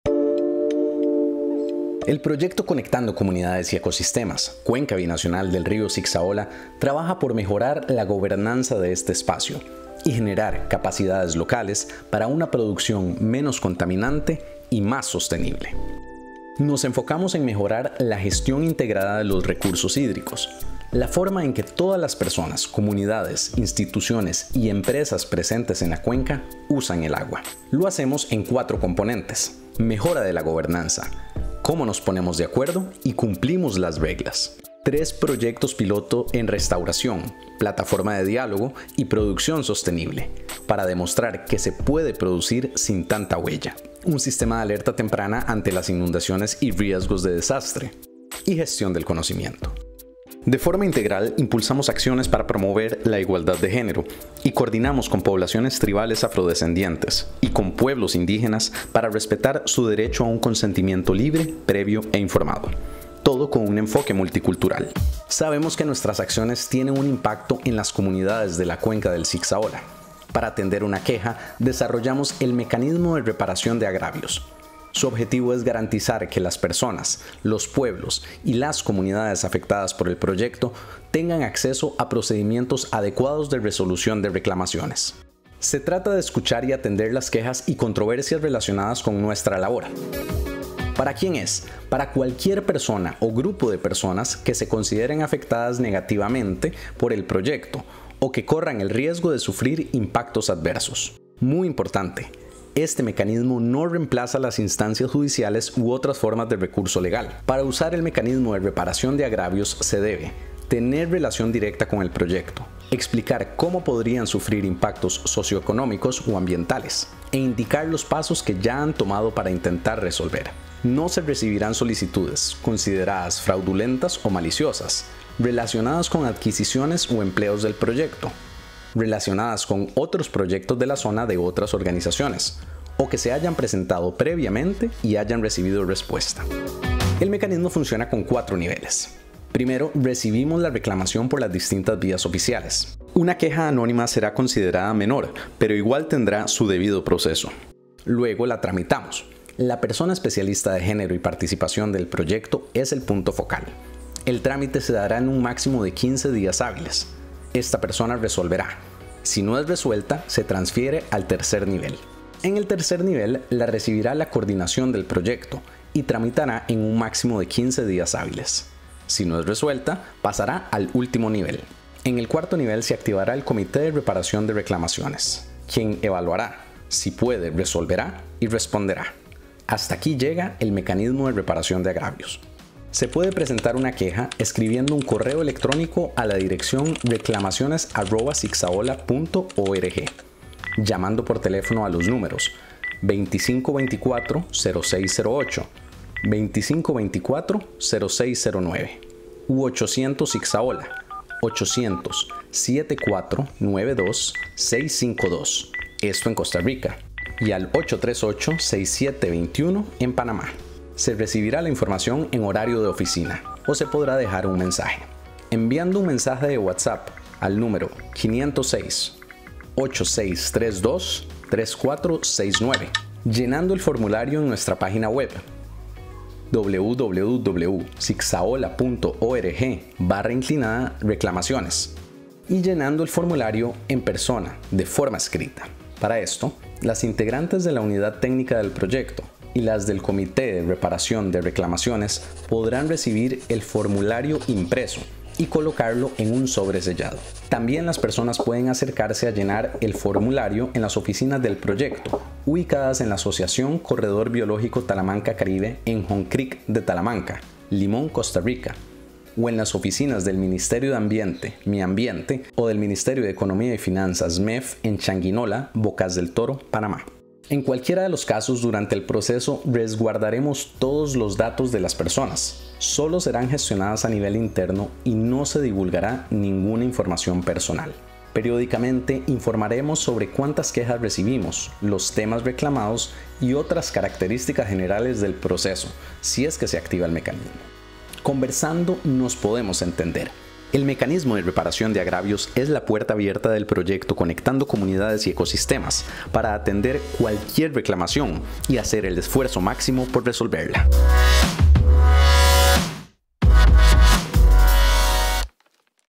¿Qué es el Mecanismo de Reparación de Agravios? - Cuenca Binacional del Río Sixaola
El Proyecto Conectando Comunidades y Ecosistemas (GEF-OET-PNUD) trabaja por mejorar la gestión del agua en la Cuenca Binacional del Río Sixaola. Nuestras acciones tienen un impacto en este territorio. Si alguna persona, grupo o comunidad se considerada afectada negativamente por nuestras labores, tienen un procedimiento adecuado para resolver reclamaciones.

El Mecanismo de Reparación de Agravios (MRA) es el camino que proponemos para escuchar y atender quejas y controversias relacionadas con las acciones del Proyecto.